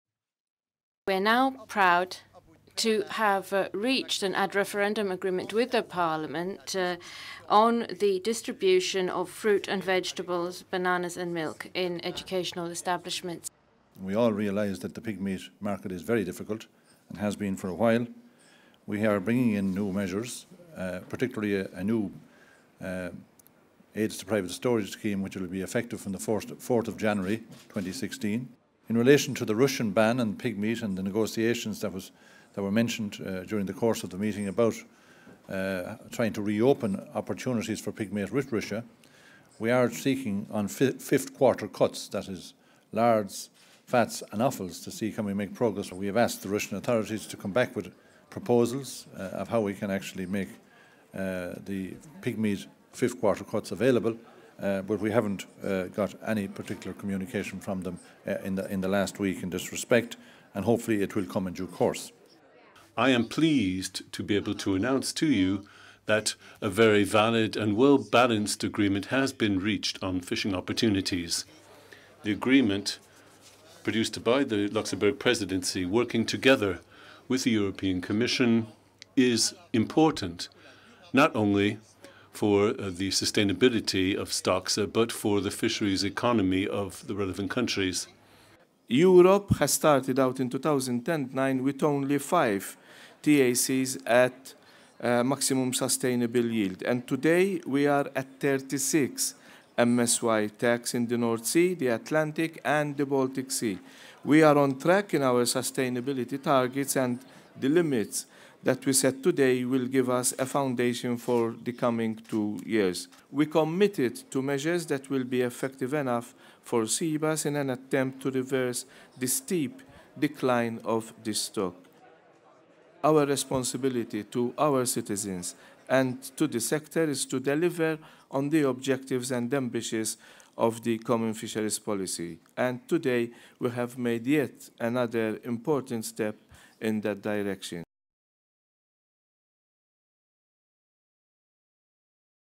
Highlights Agriculture and Fisheries Council
Highlights of the Agriculture and Fisheries Council, on 14-15 December 2015, in Brussels. Ministers reached an agreement on the 2016 fishing opportunities for the stocks in the Atlantic, the North Sea, international fisheries in which EU vessels participate and in the Black Sea. Ministers also reached a tentative agreement with the European Parliament on a proposal on school fruit, vegetable and milk scheme.